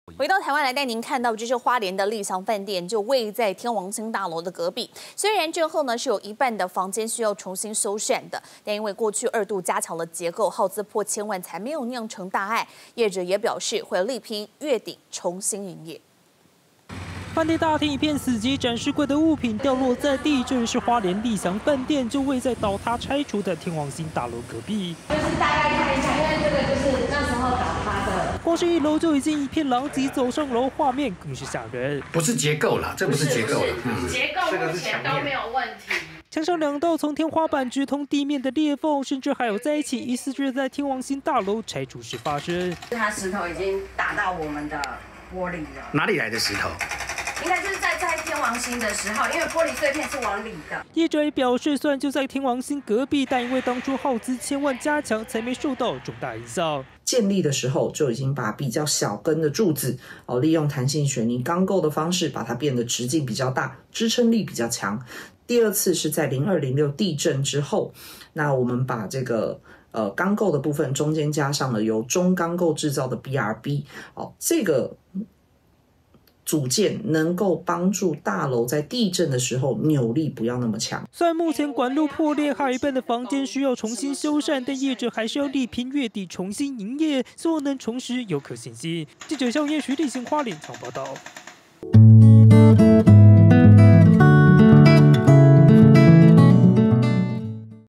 麗翔飯店震後修繕「破千萬」！ 業者拚月底「重新營業」
花蓮麗翔飯店，就位在倒塌拆除的天王星大樓隔壁，雖然震後到現在，還有一半的房間需要重新修繕，但因為過去二度加強結構，耗資破千萬才沒有造成大礙，業者也表示要力拼月底重新營業，讓遊客重拾對花蓮觀光的信心，但在這之前就已經損失破千萬元。